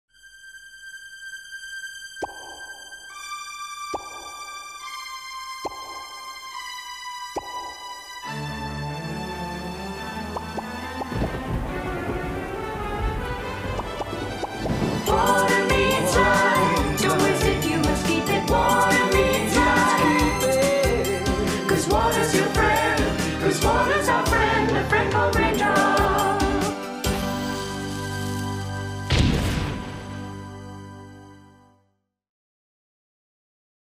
𝗥𝗖𝗘'𝘀 𝗠𝗼𝘁𝗶𝗼𝗻 𝗣𝗶𝗰𝘁𝘂𝗿𝗲𝘀 𝗦𝘂𝗿𝗽𝗿𝗶𝘀𝗲: 𝗥𝗮𝗶𝗻𝗱𝗿𝗼𝗽 - 𝗪𝗮𝘁𝗲𝗿 𝗶𝘀 𝗔𝗱𝘃𝗲𝗻𝘁𝘂𝗿𝗲 𝗶𝘀 𝗻𝗼𝘄 𝗼𝗻 𝘁𝗵𝗲 𝗜𝗻𝘁𝗲𝗿𝗻𝗲𝘁 𝗔𝗿𝗰𝗵𝗶𝘃𝗲!
𝗣𝗿𝗼𝗺𝗼 𝗘𝗱𝗶𝘁𝗲𝗱 𝗕𝘆: L.N. Andrews (@northernplacecorporation)

L.N. Andrews' newest LSN has arrived! Today, he's profiling the series Raindrop - Water is Adventure! Make sure you stick around for NPC's first Millennideo! Also, life is like a box of chocolates.